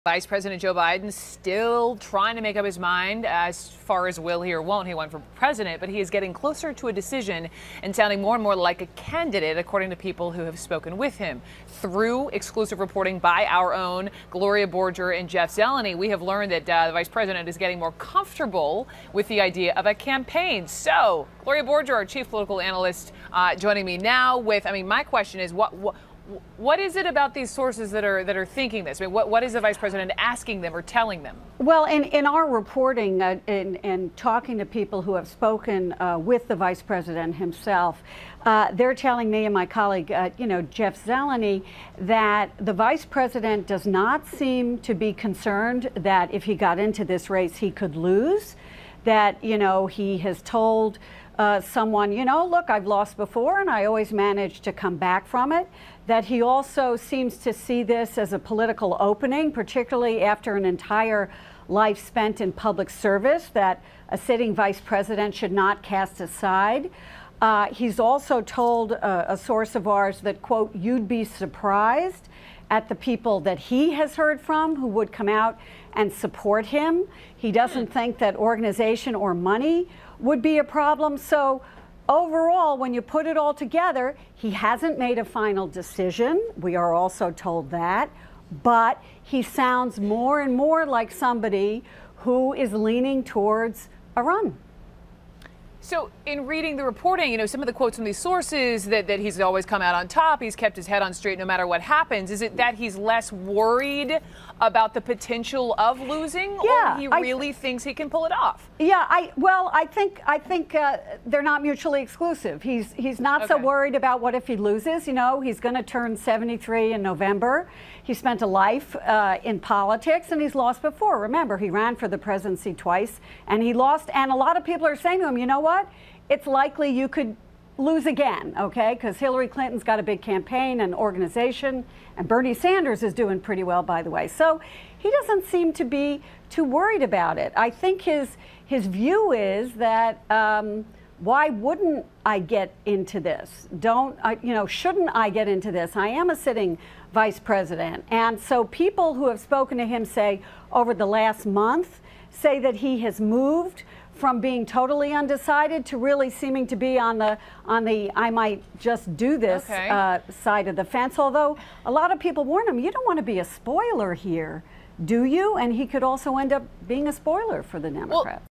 Friends: Joe Biden closer to a 2016 decision
CNN's Gloria Borger reports that friends of Joe Biden say he is sounding more like a candidate and is coming closer to a decision on a 2016 run.